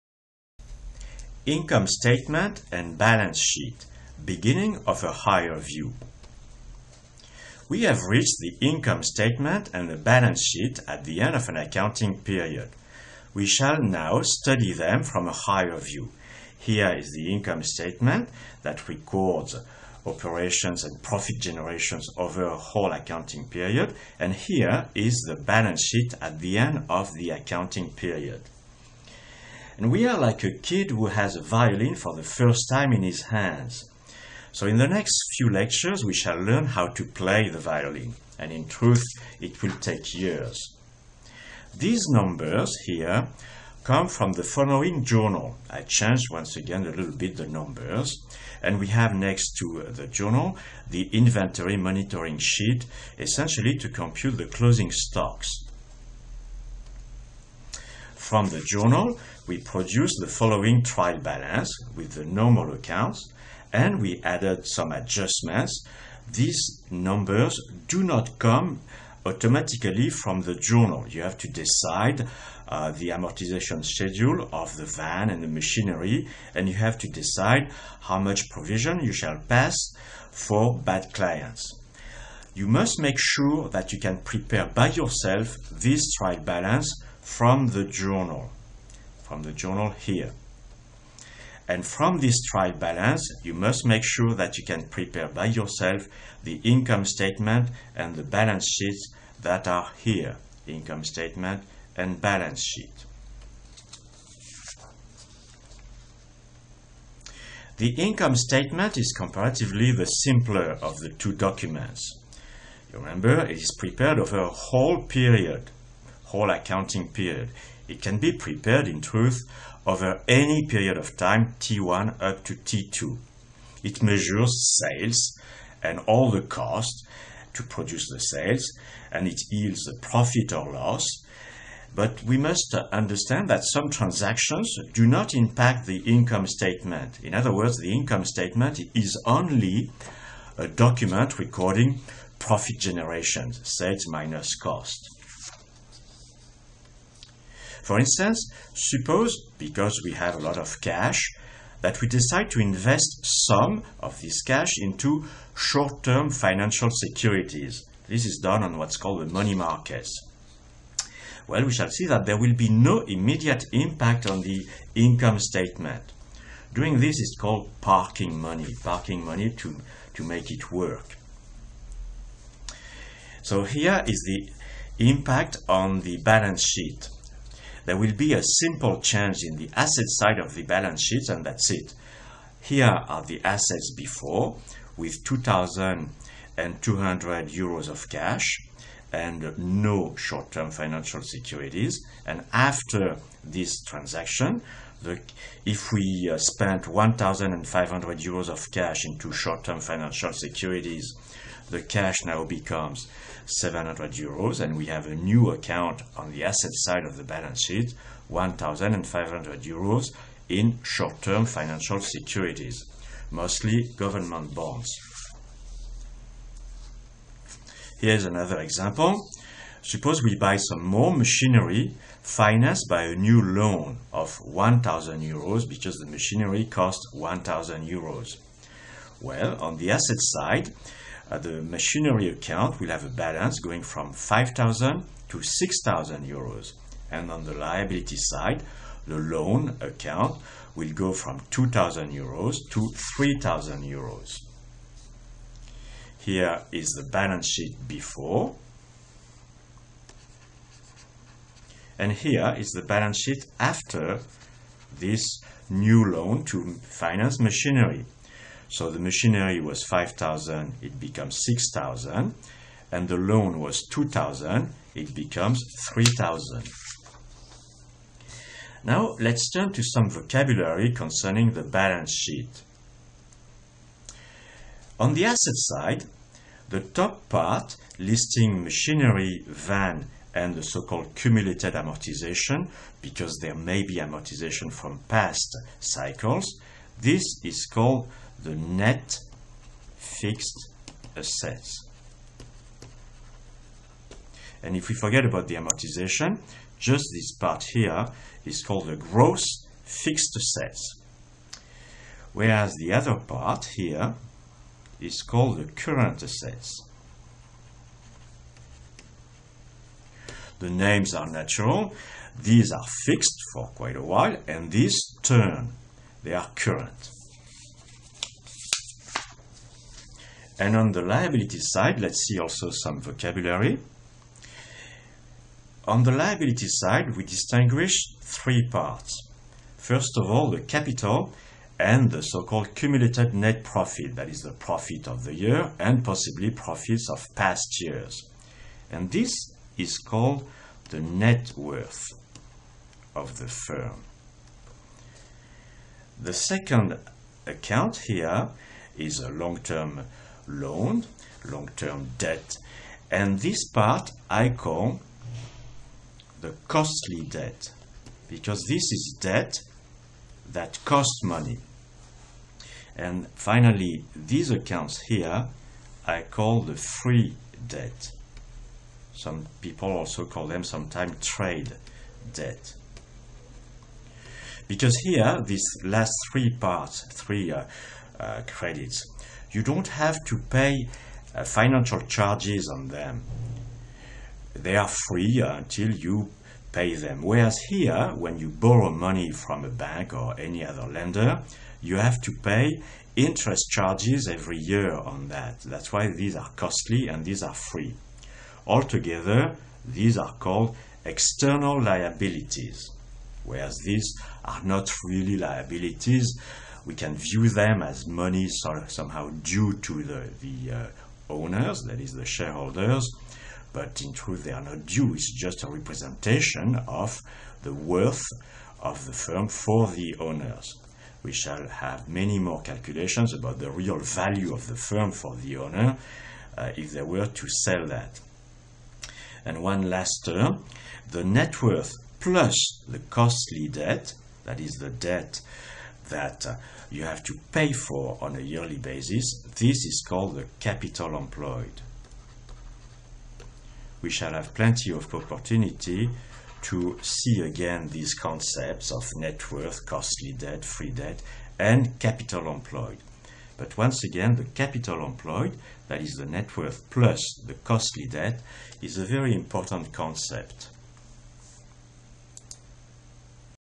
income statement and balance sheet: beginning of a higher view
income statement and balance sheet: beginning of a higher view. Transactions which do not affect the IS. Vocabulary: fixed assets, current assets, net worth, costly debts, free debts.